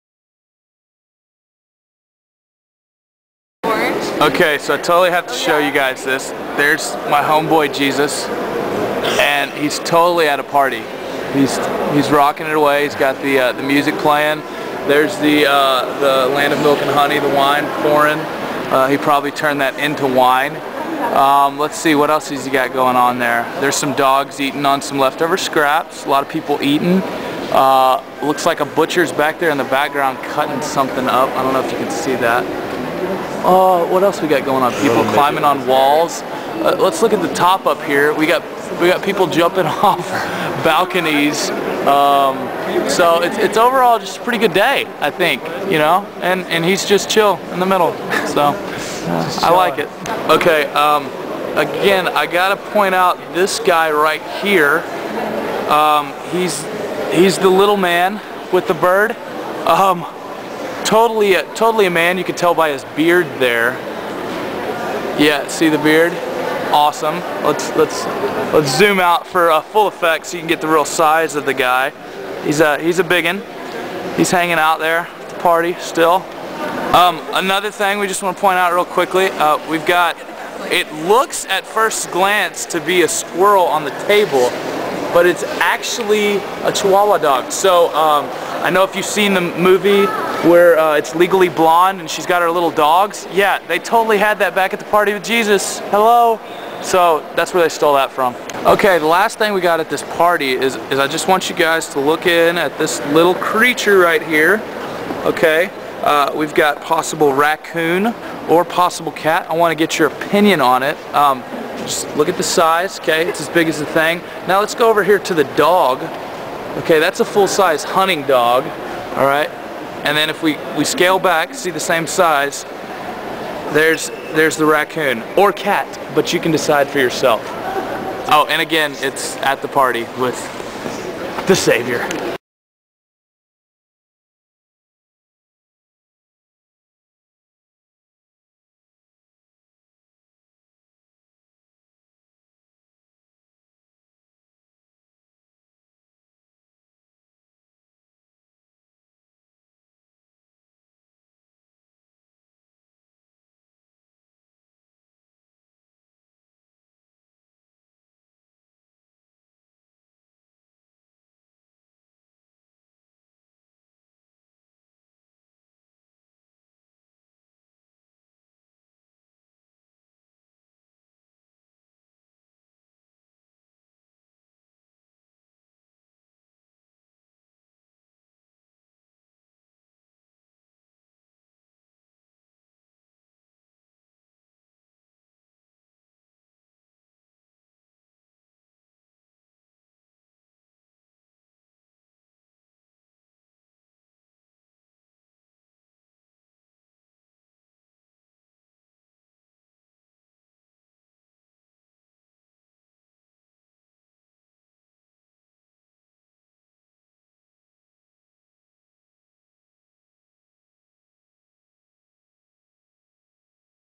Wedding at Cana
The coolest painting in the Louvre. A depiction of the wedding at Cana from 16th century painter Paolo Veronese. It depicts the first miracle of Jesus. Watch as I discover some cool things in the painting. Also notice around the :23 or :24 just how big the painting is. Jesus throws down at Cana.